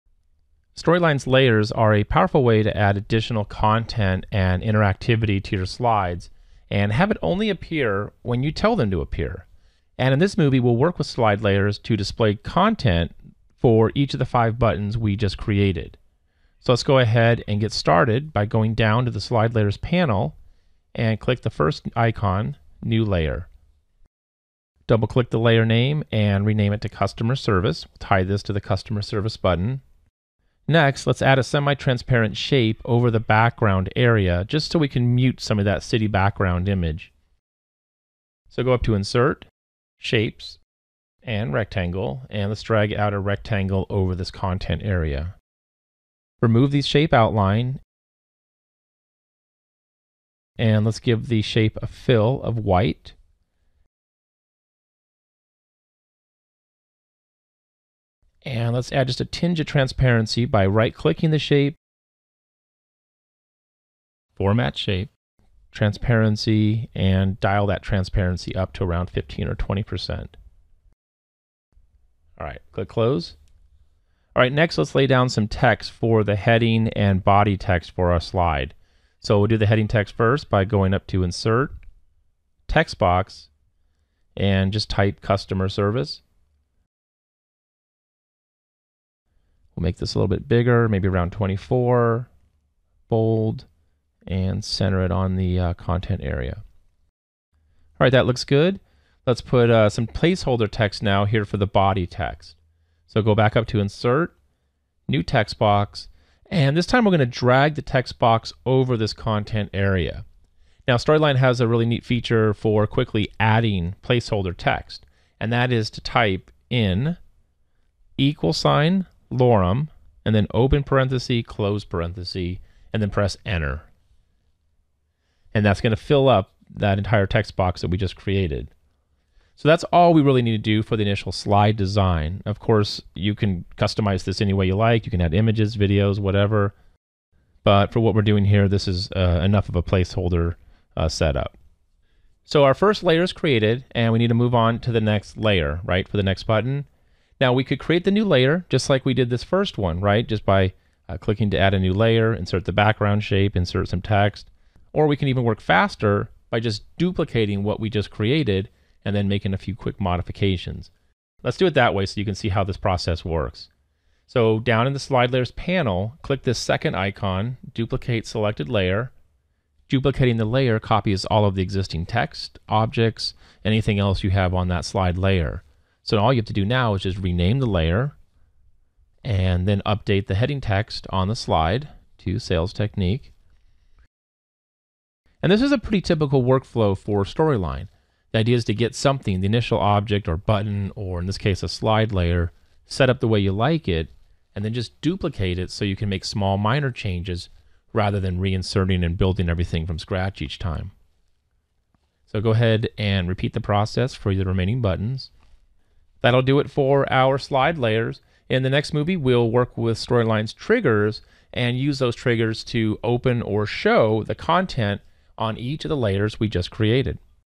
Articulate Storyline 360: Creating a Tabs Interaction (Adding Slide Layers)
Now that we have tabs set up for our interaction, we just need to create a layer with additional content for each tab.